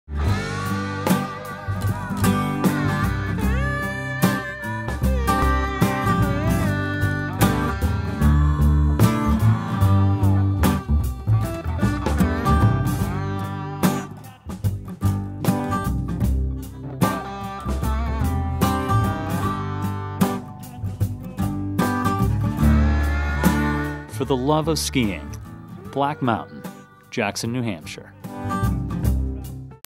Black Mountain, Jackson NH - 2012 :30 Second Commercial
Black Mountain in Jackson, New Hampshire offers families and skiers the best that Mount Washington Valley has to offer: versatile terrain, the best value for your dollar in the region, we're family and community focused, and if you haven't been to the Lostbo Pub for aprés ski, you're in for a real treat when you feel the cheerful ambiance and warm atmosphere.

For the love of skiing. Black Mountain. Jackson New Hampshire.